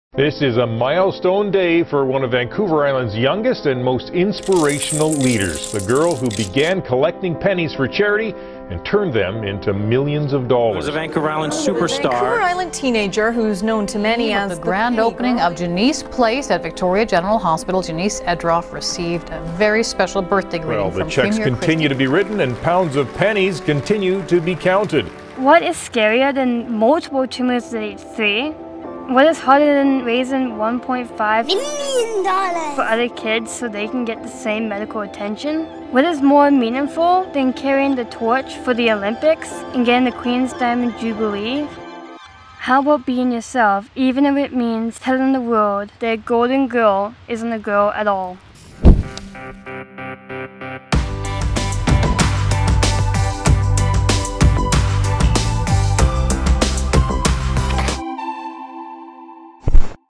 PENNY GIRL
Small change is a big deal. Just ask the kid who raised $1.6m in pennies, beat cancer, and recently came out as trans.